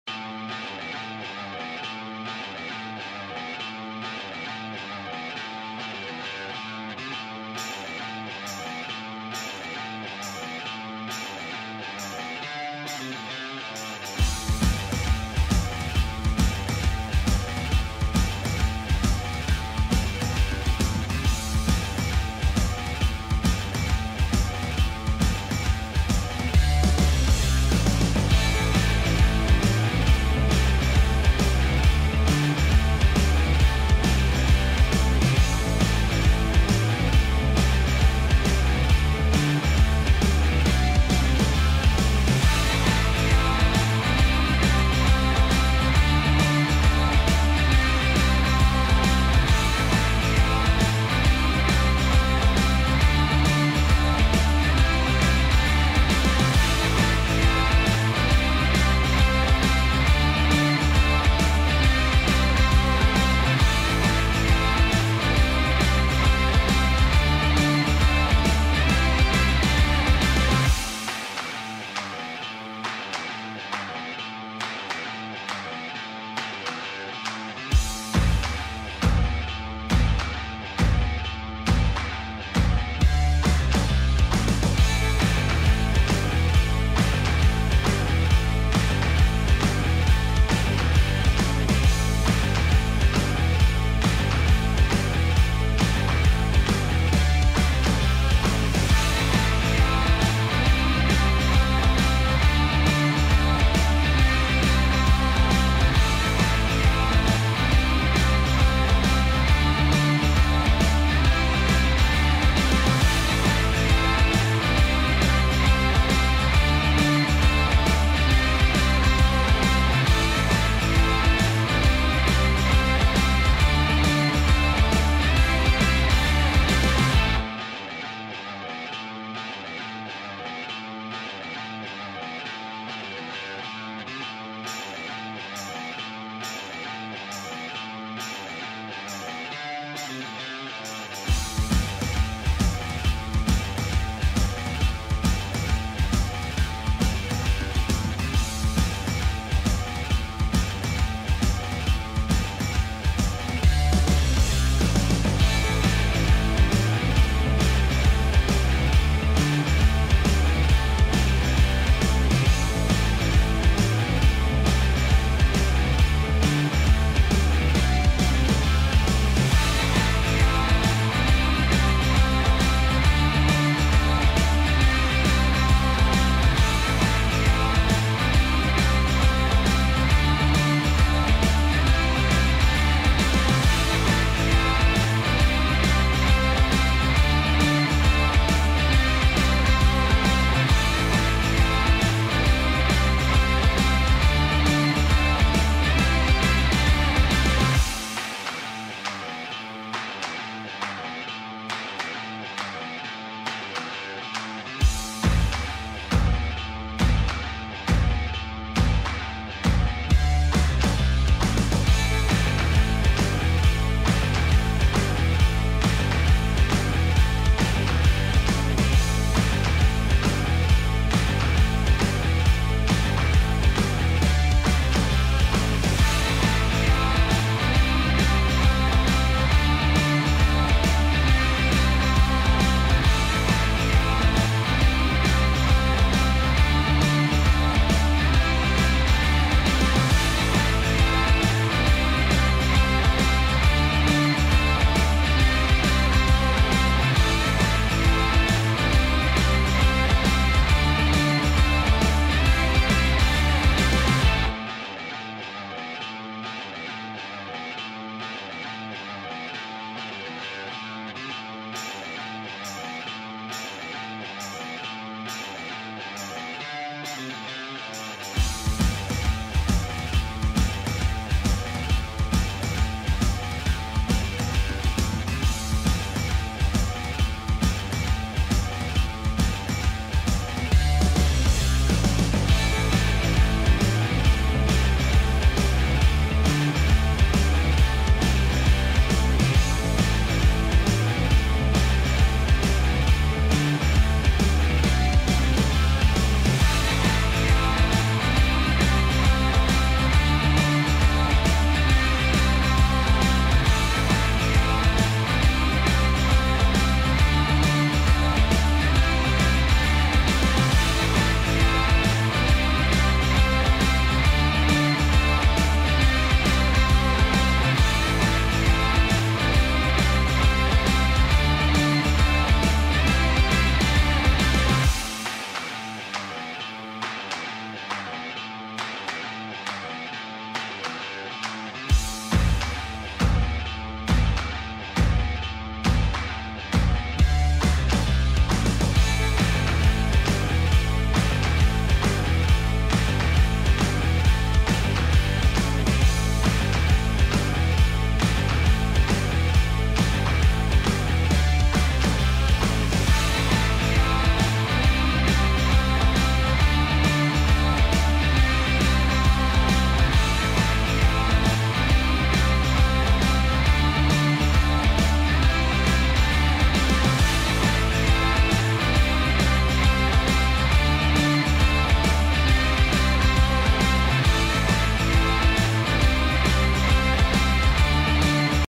Tracking multiple targets in 'infrared' | AI Computer Vision Target Tracking Tech
I made a tracking system using a simulated infrared video feed. This is a demo of a basic object tracker, using infrared imaging as input to track 'hot' objects. After a target is selected, the tracking is autonomous. Multiple targets can be selected, all with independent tracking. This demo is on a simulated unstable hovering camera source to test the tracking software under erratic motion.